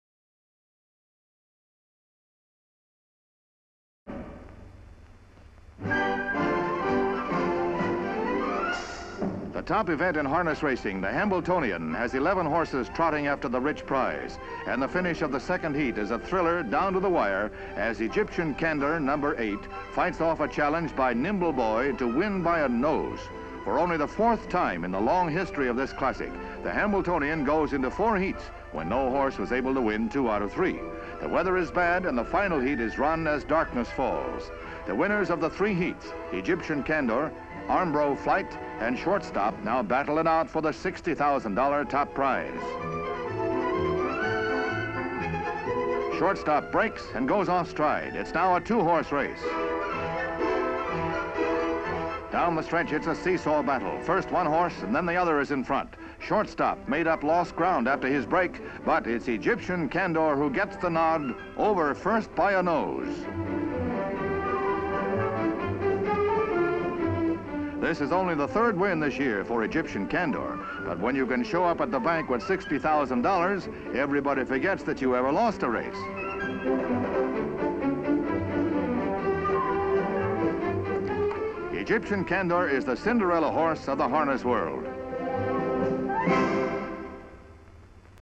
Trotters participate in the race and Egyptian Candor wins the race in Du Quoin, I...HD Stock Footage
Trotters participate in the race and Egyptian Candor wins the race in Du Quoin, Illinois.

Harness racing in Du Quoin, Illinois. Sign:'Welcome to Du Quoin' and 'Home of to Hambletonian'. Spectators watch the harness racing. Trotters participate in the race as they run on race course. "Egyptian Candor" (Trotter)wins the race and a prize of $60,000. Sign: 'Victory Lane' in the background. Location: Du Quoin Illinois. Date: September 6, 1965.

57,000+ broadcast-quality historic clips for immediate download.